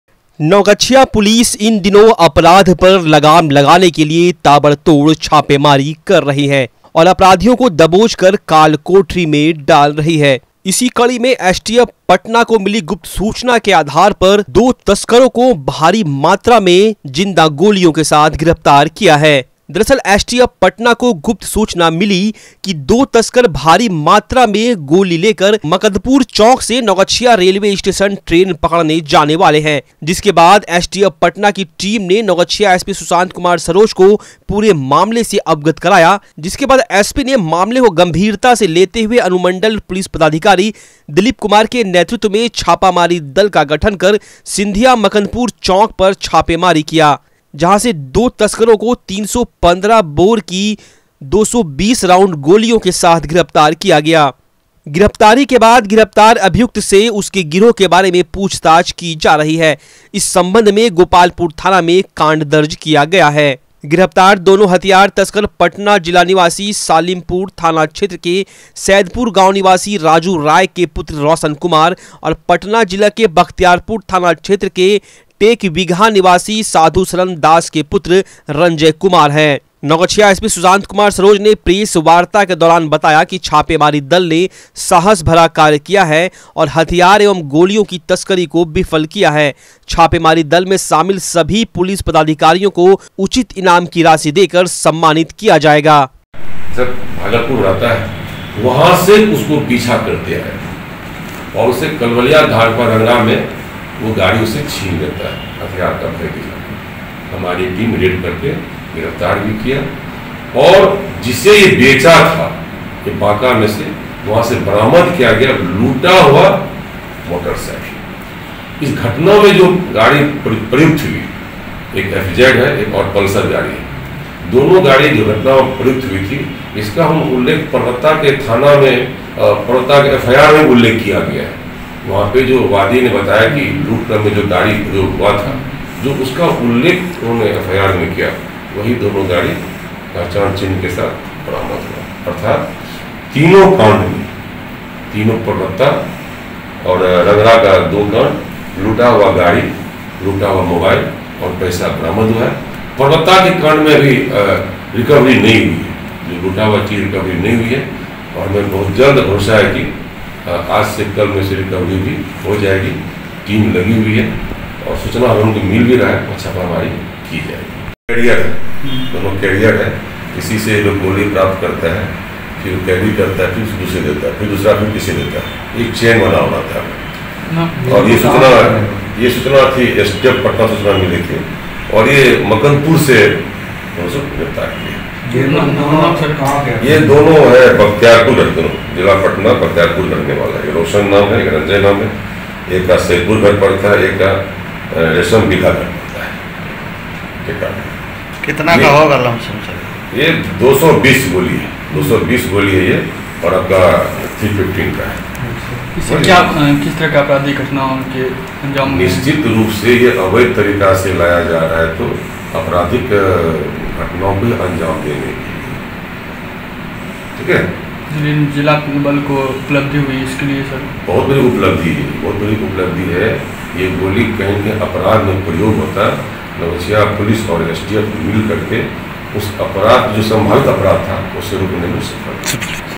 Naugachiya : पुलिस की बड़ी सफलता, भारी मात्रा में कारतूस के साथ दो तस्कर गिरफ्तार | Bihar News
एसटीएफ पटना को गुप्त सूचना मिली कि- दो गोलियों तस्कर भारी मात्रा गोलियों लेकर मकदपुर चौक से नवगछिया रेलवे स्टेशन ट्रेन पकड़ने जाने वाला है।जिसके तत्पश्चात एस०टी०एफ० पटना की टीम ने नवगछिया एसपी सुशांत कुमार सरोज को पूरे मामले से अवगत करवाया।जहाँ नवगछिया एसपी सुशांत कुमार सरोज ने मामले को गंभीरता से लेते हुए, अनुमंडल पुलिस पदाधिकारी दिलीप कुमार के नेतृत्व में  छापामारी दल का गठन कर सिंधिया मकंदपुर चौक पर छापामारी किया गया. जहाँ दो व्यक्ति को 315 बोर का 220 राउंड गोलीयां के साथ गिरफ्तार किया गया।